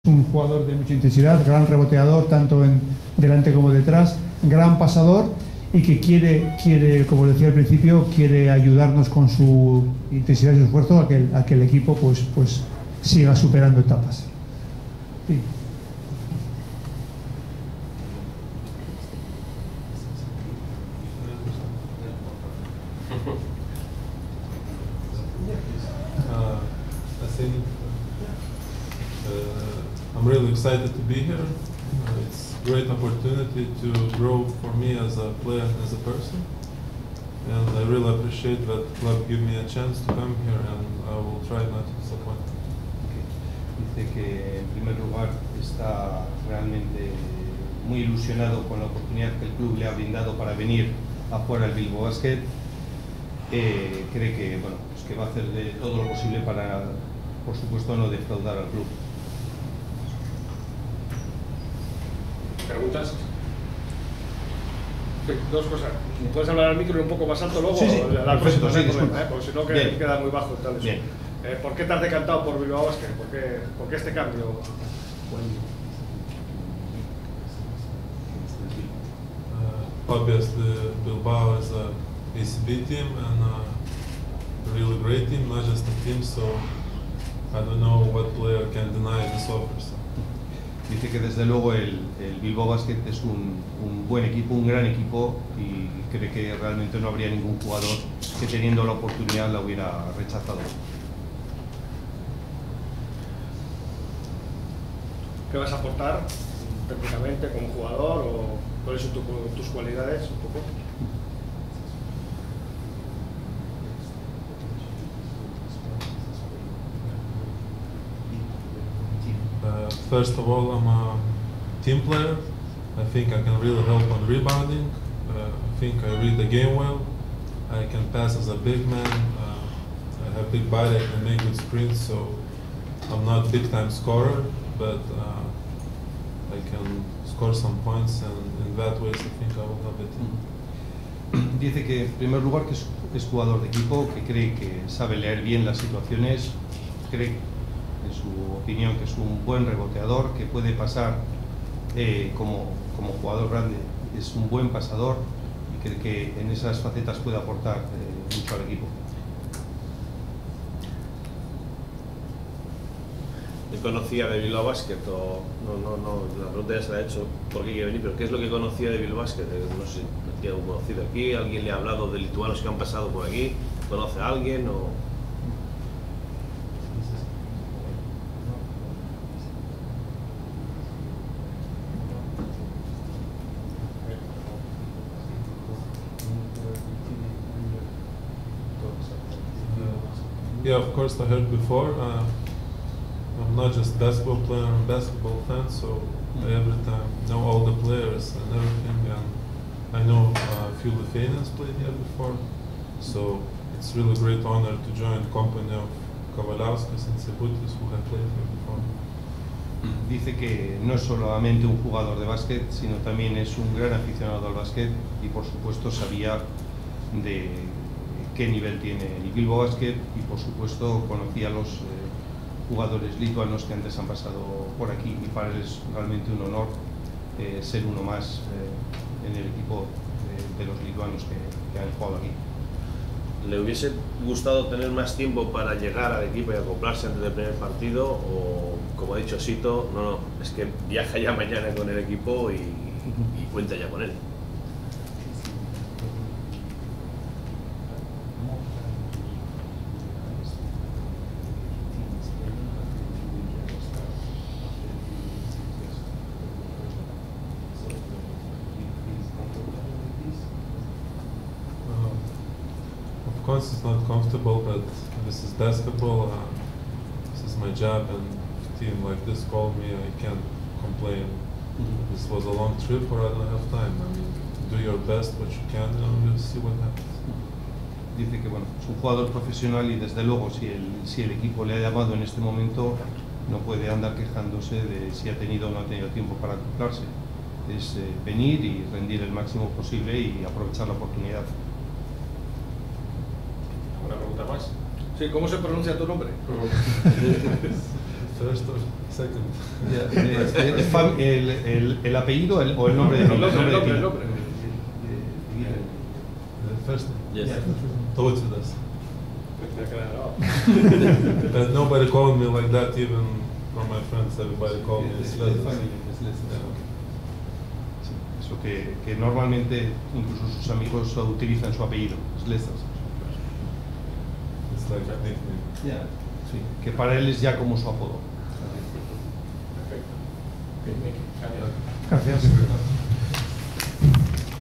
Presentación de Tautvydas Slezas como jugador del Dominion Bilbao Basket
El jugador lituano se estrenó en Sala de Prensa como jugador del Dominion Bilbao Basket